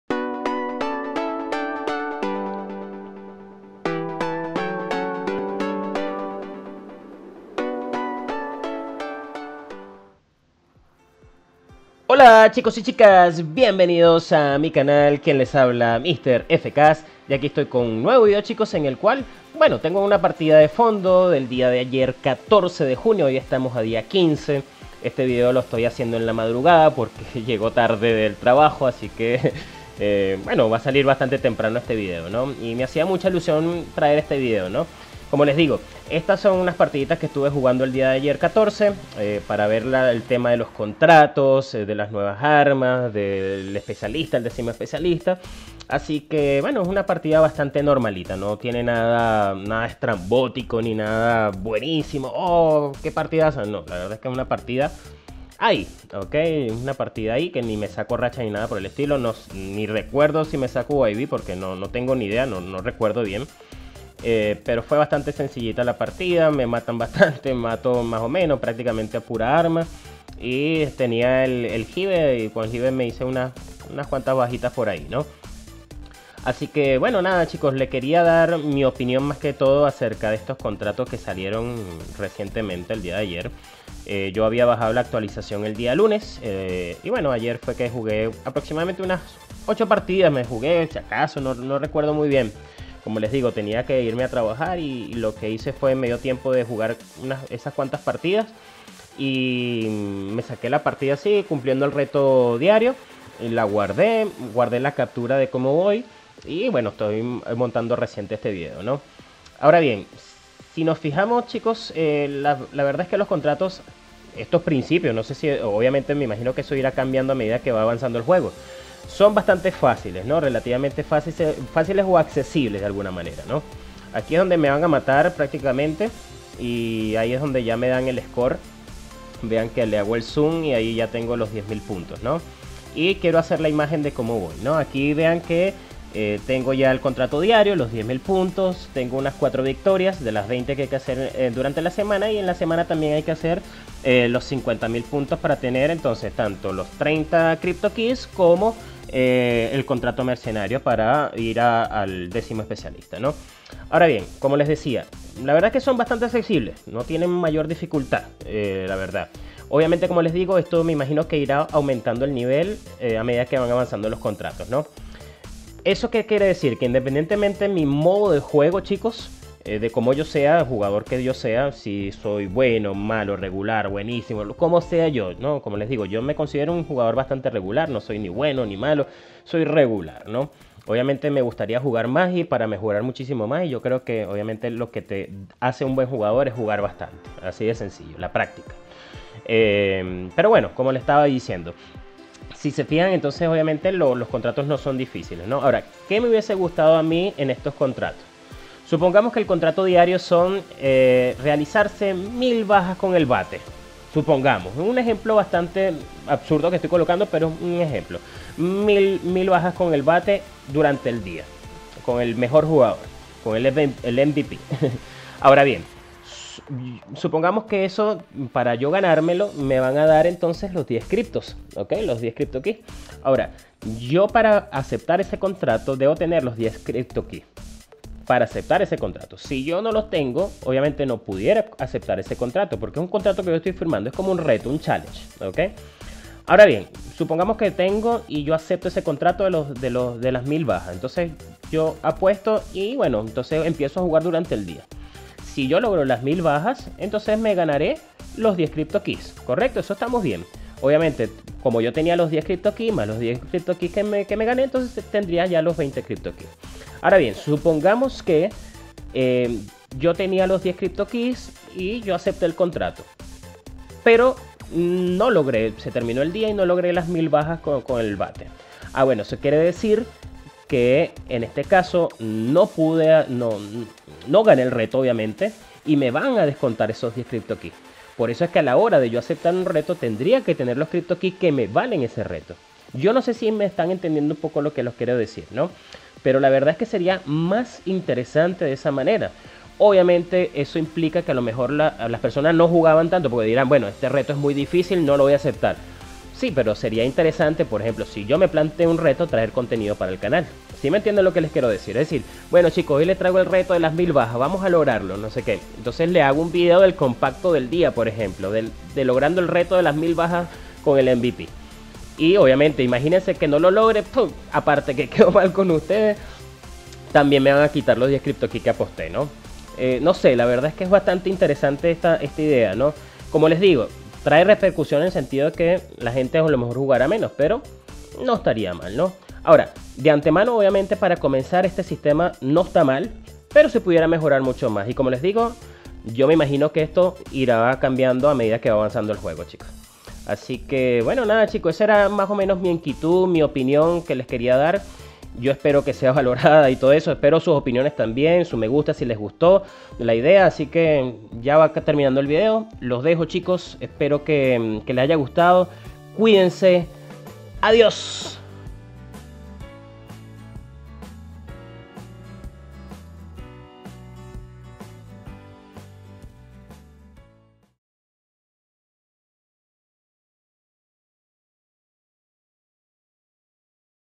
Contratos en Black Ops 3, mi opinión, ¿Pueden mejorar?
Hola chicos y chicas, bienvenidos a mi canal, En el presente vídeo les comento mi opinión sobre cómo pudieran mejorar los contratos en Black Ops 3. Gracias.